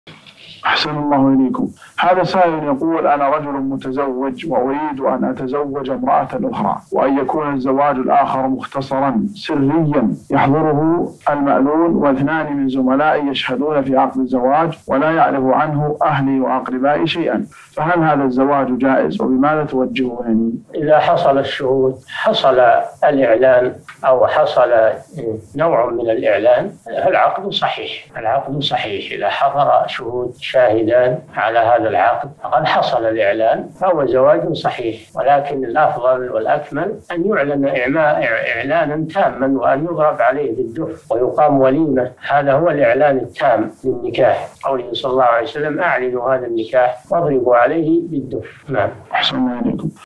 أريد أن أتزوج الثانية ولكن دون علم أهلي وأقربائي ، فهل يجوز هذا الزواج؟ الشيخ صالح بن فوزان الفوزان
نص السؤال: أنا رجل متزوج، وأريد أن أتزوج امرأة أخرى، وأن يكون الزواج الآخر مختصراً، سريًّا، يحضره المأذون واثنان من زملائي يشهدون في عقد الزواج، ولا يعلم عنه أهلي وأقربائي شيئاً، فهل هذا الزواج جائز، وبماذا توجهونني؟
السيرة الذاتية :
للاشتراك في قناة الشيخ صالح الفوزان

رئيس الفريق
لمشروع كبار العلماء